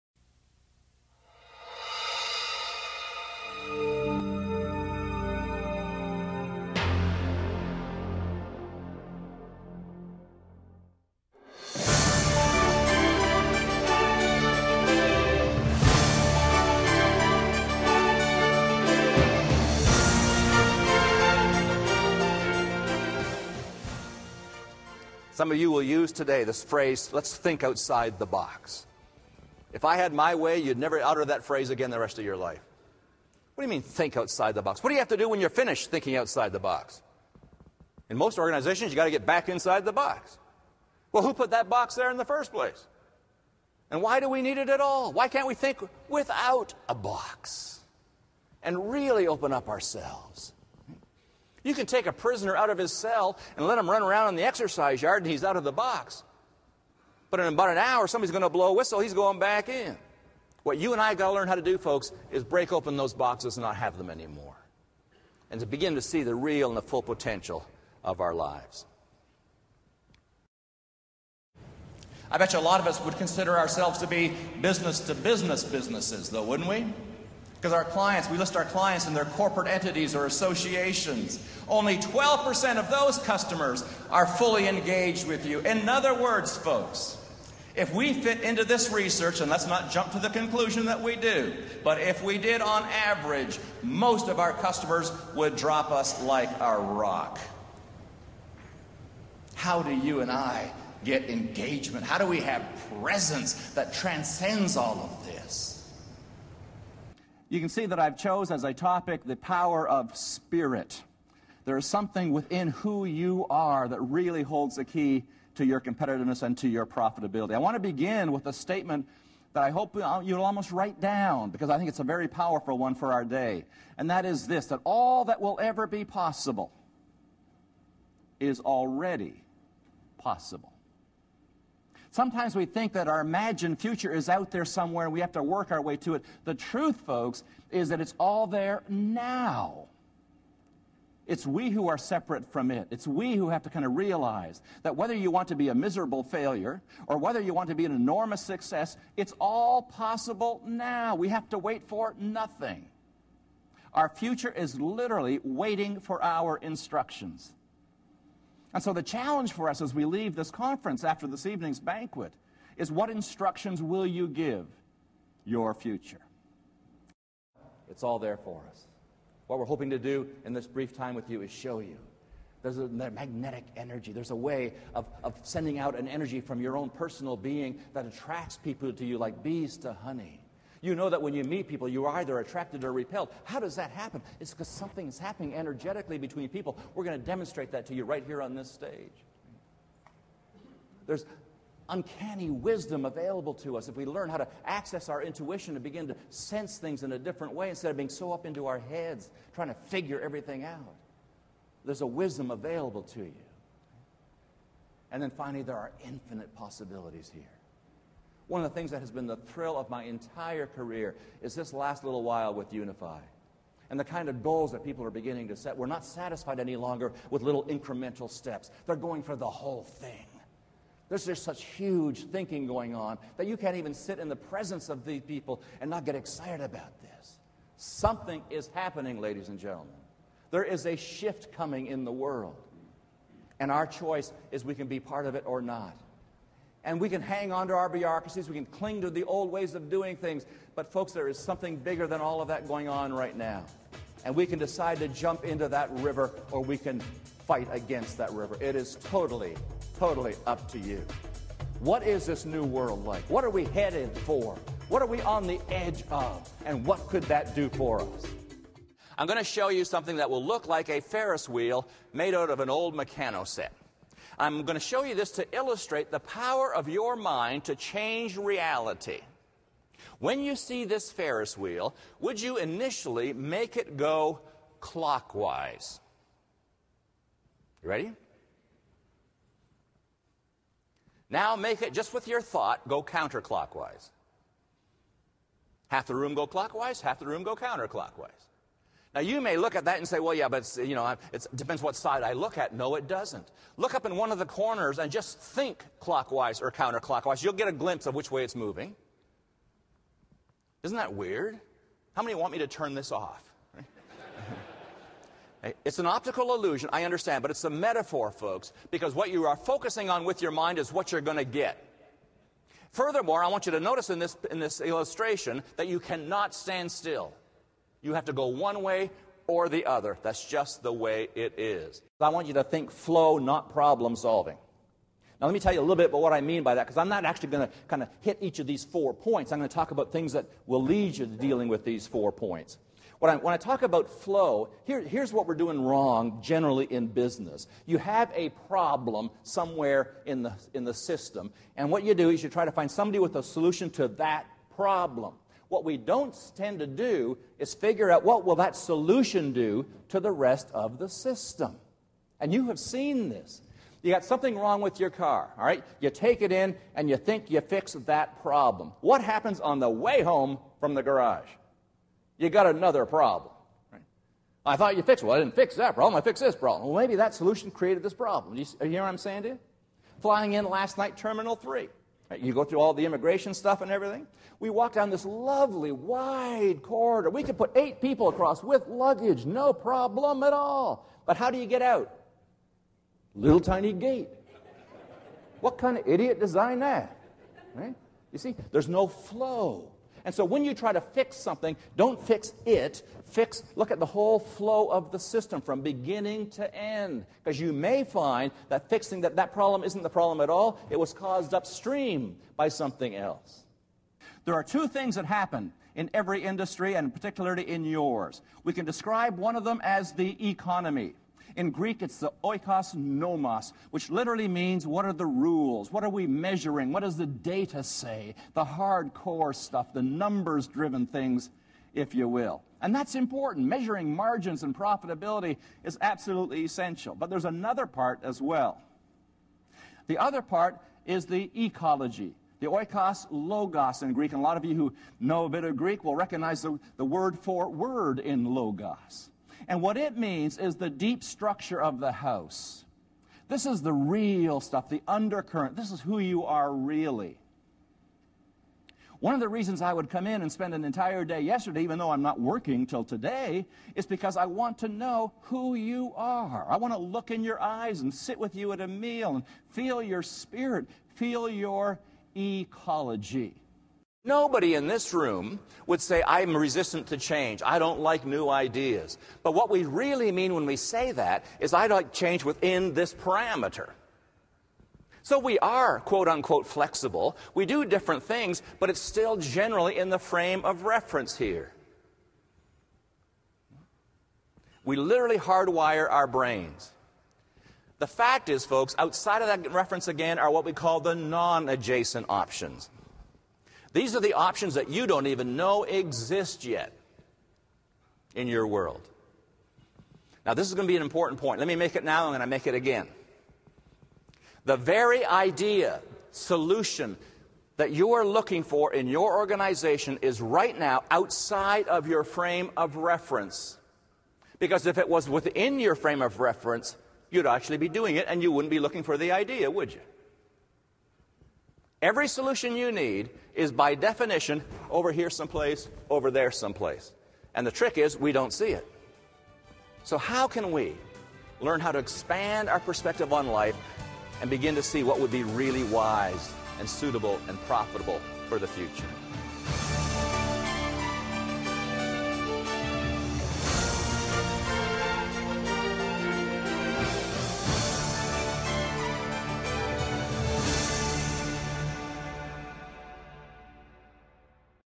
Ian Percy, HoF, CPAE - Our newest Keynote Sampler -
Unassailable success comes when the organization aligns all its energies and focuses them on the highest possibilities. Have Ian speak at your next event.